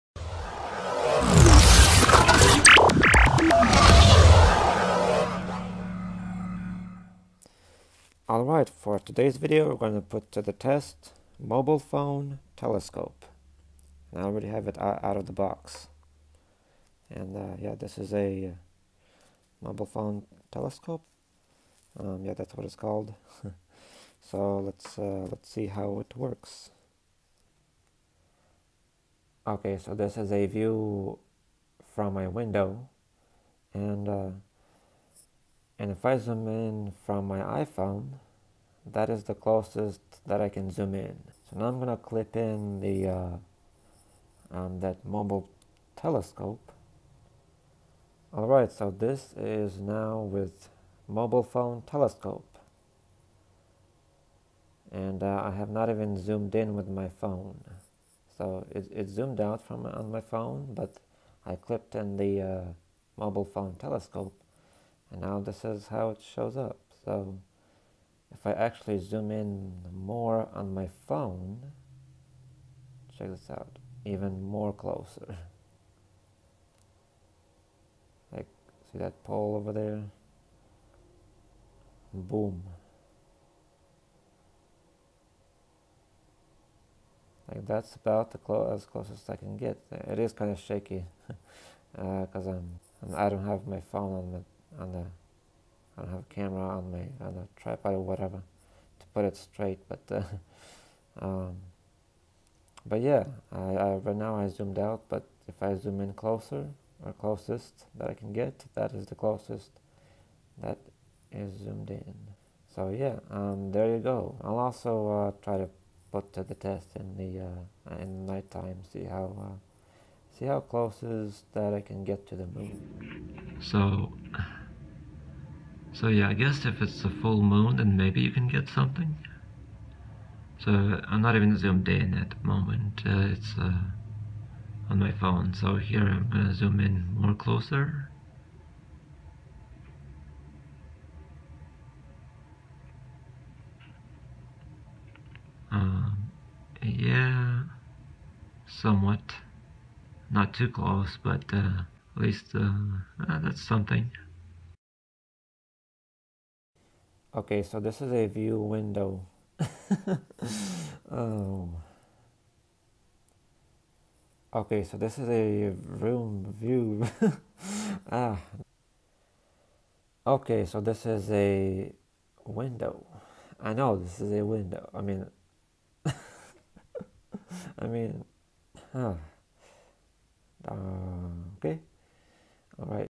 Mobile Phone Optical Zoom Telescope Put To The Test
Just found a pretty cool product, mobile phone optical zoom telescope, decided to get it and put it to the test and see how it actually works, I may do another update video on it in the near future.